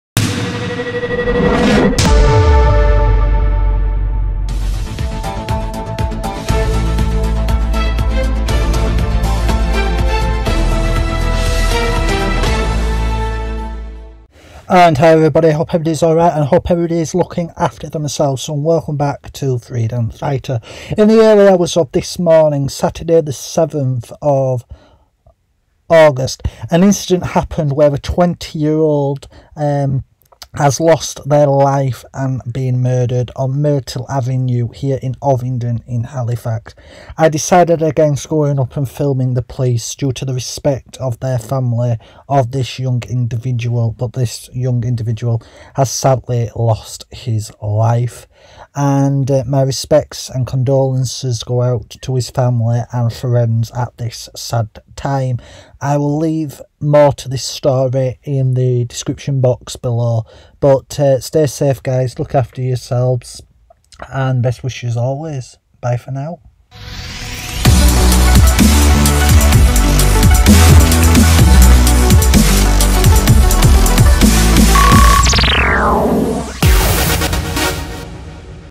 Man in his 20s 'murdered' in Myrtle Avenue, Halifax, as police launch major investigation
Out of the respect of his family I did not go up to Myrtle Avenue this morning to record west Yorkshrie police we have some sad news that a Man in his 20s 'murdered' in Myrtle Avenue, Halifax, as police launch major investigation

Here at Freedom Fighter our Condolences go out to his family and friends and loved ones